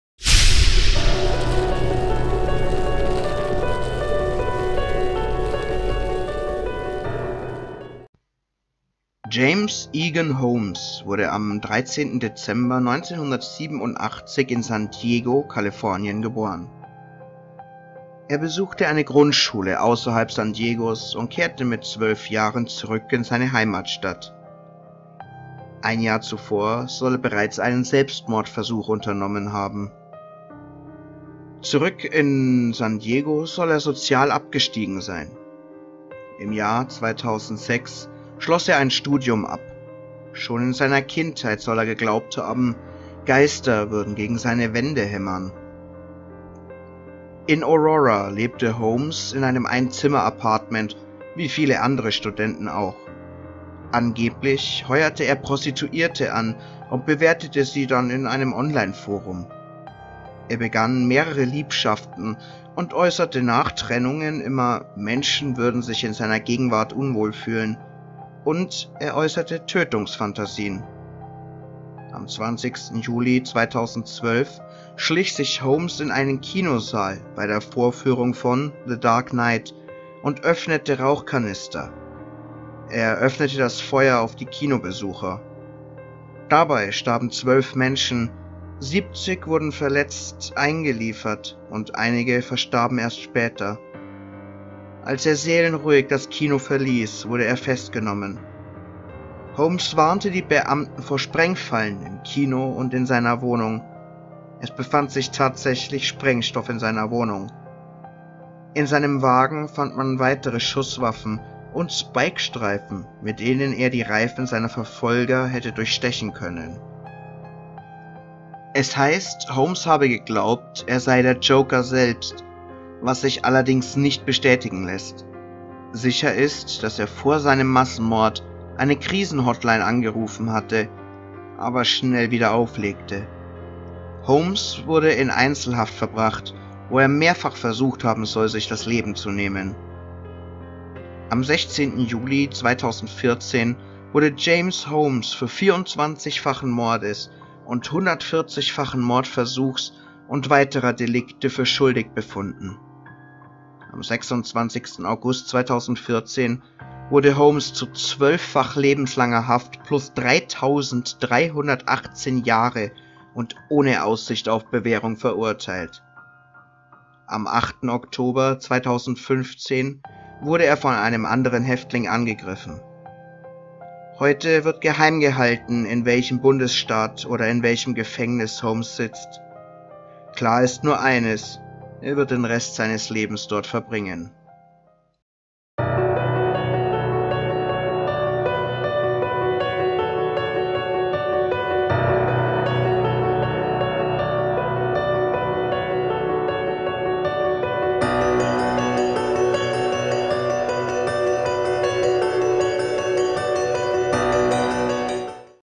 Mörderakte: #155 James Holmes / Mystery Detektiv
Thumbnails ab Mörderakte 247 Ich selbst James Holmes ist bekannt für das Massaker, dass er anrichtete und für seine auffälligen Haare.